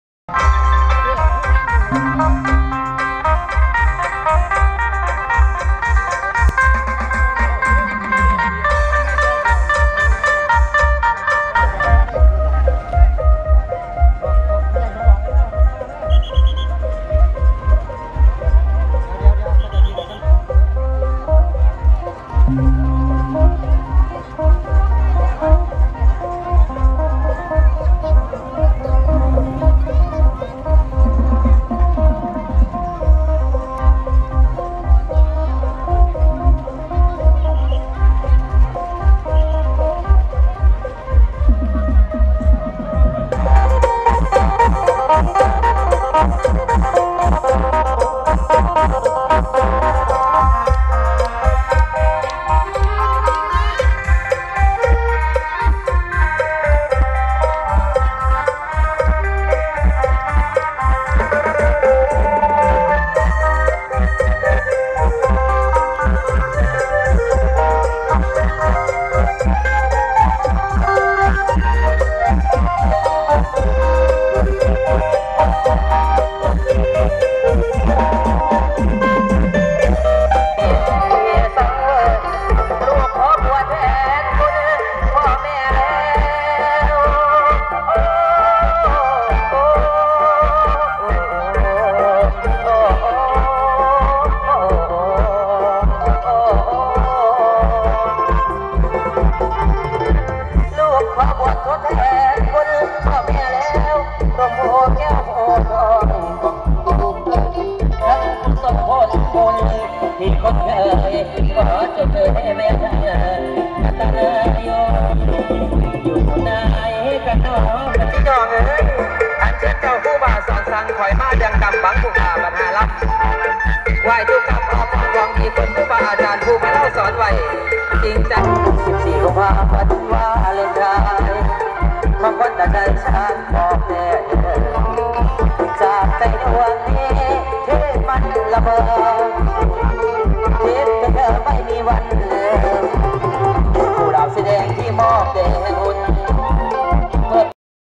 เทพคีรี แห่ผเวส
บุญผเวส หรือ บุญพระเวสสันดร ที่ชาวบ้านเทพคีรี ได้ปฏิบัติสีบทอดมาทุกปี ปีนี้ทำบุญตรงกับวันที่24มีนาคม 2557 ทำพิธี ที่ข้างลำห้วยพะเนียง ใกลัสะพานข้ามไปบ้านวังสามหาบ แล้วแห่เข้าสู่บ้านเทพคีรี   ณ วัดเทพนิมิตร